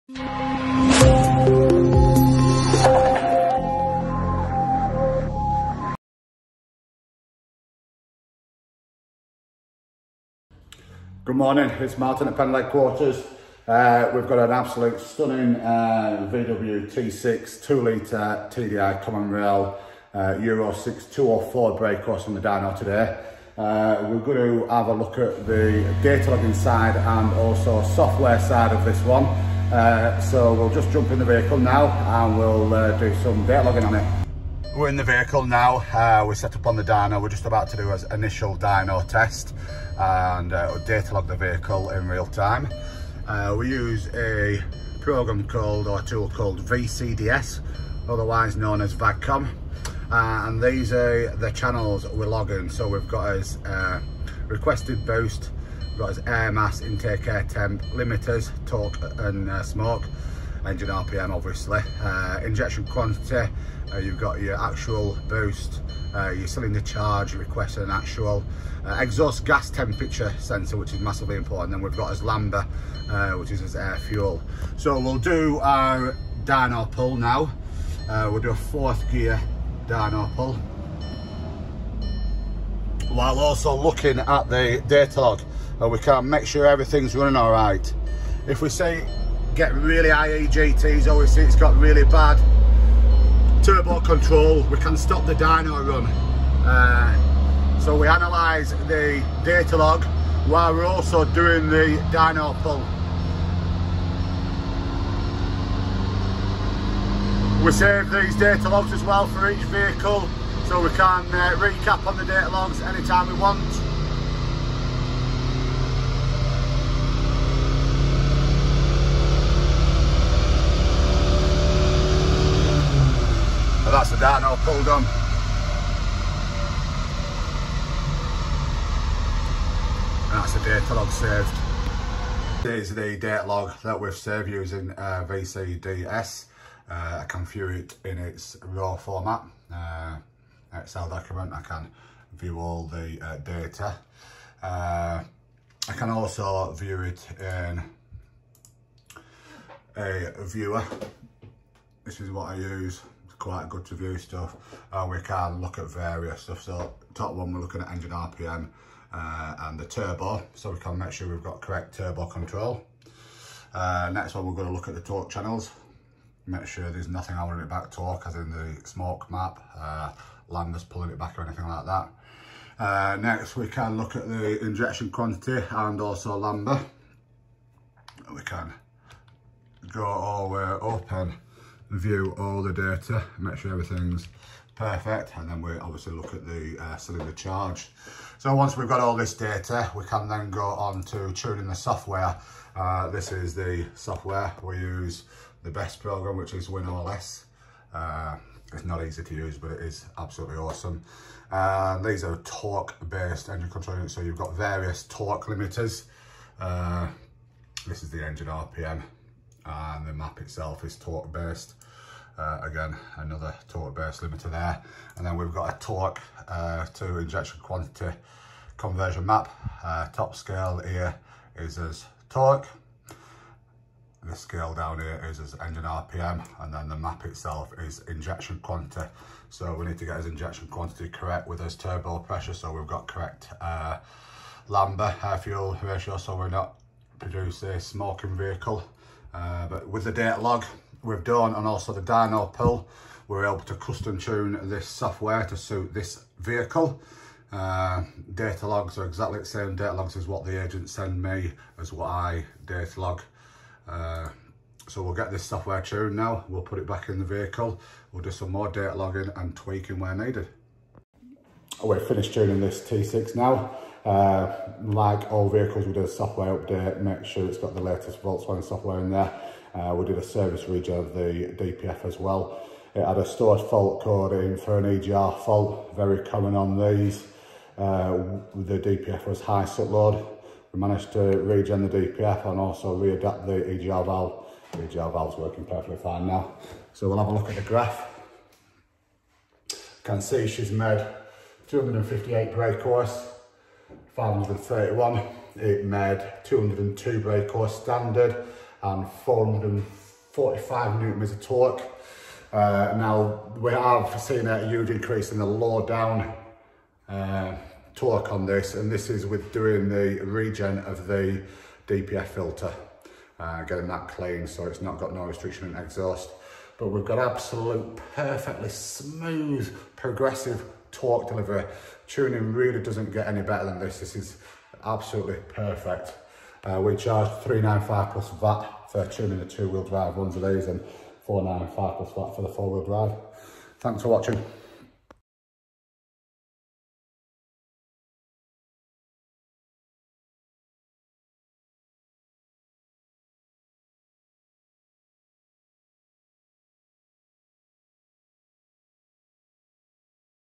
T6 Transporter (2.0 TDI-CR 204bhp Euro6) Datalogging & Tuning | Pendle Performance
Watch as Martin at Pendle Performance’s Lancashire HQ performs datalogging and Pendle Performance bespoke ECU tuning on a VW Transporter T6 (2.0-litre TDI Common Rail 204bhp Euro6).

The datalogging is performed using the VCDS software (‘VAG-Com’) and is a vital process for analysing each vehicle prior to any upgrade and tuning work being carried out.

Once it has been ascertained that the vehicle is healthy, it can then be given a bespoke ECU calibration on the MAHA dyno.

To find out more about Pendle Performance and find the nearest Pendle-approved agent near you, head over